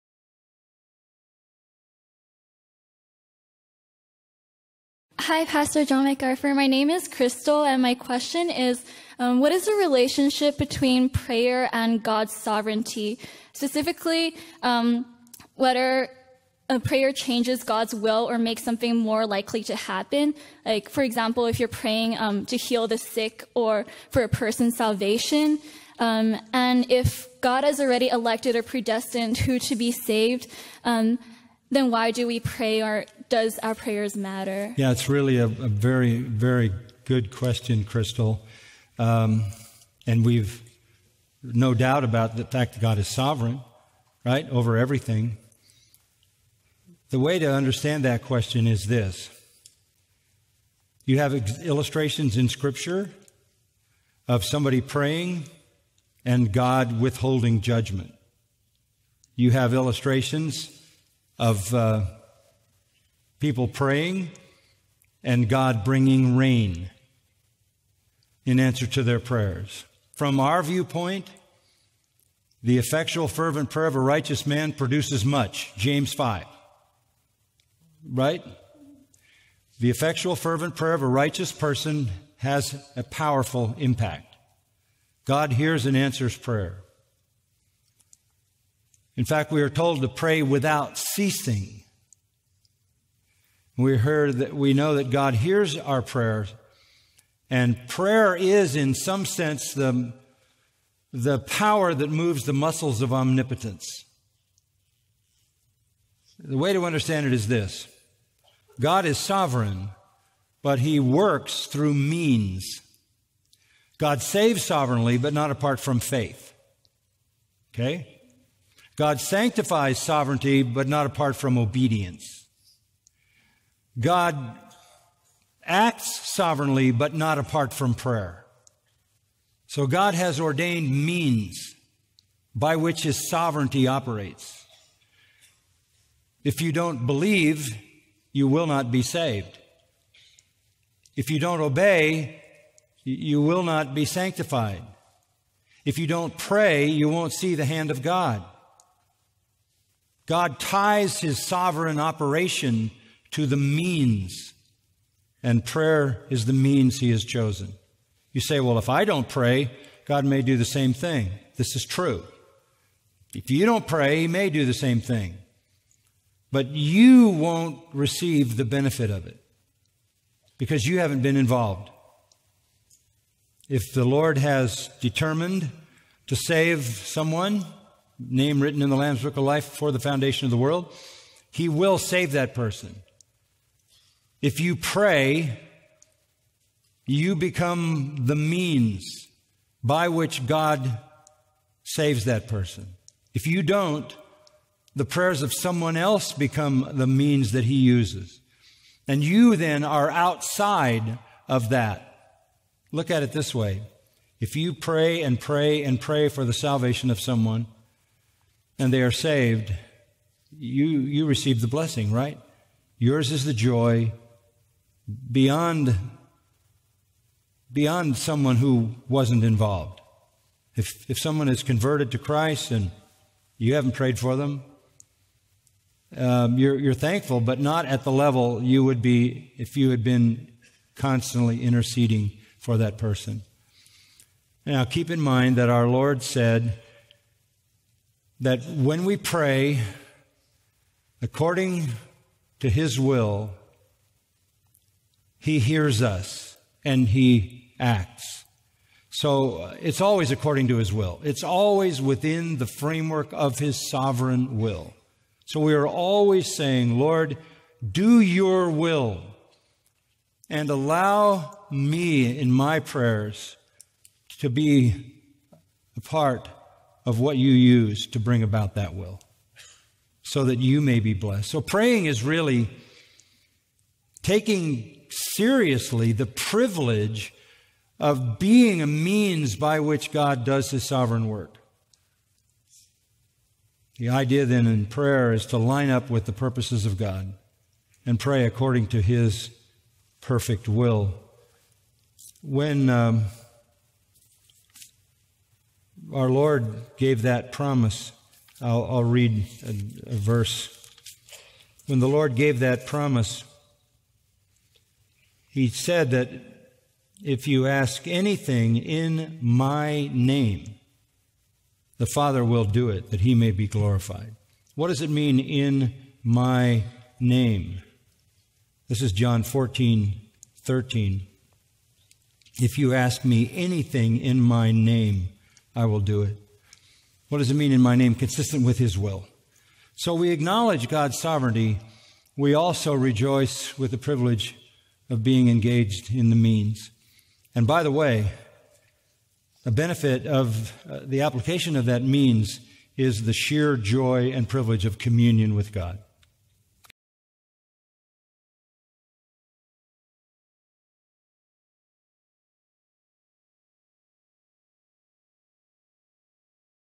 祷告与神的主权有什么关系呢？（简体版）
剪辑自约翰．麦克阿瑟的「圣经答问大会-第71次」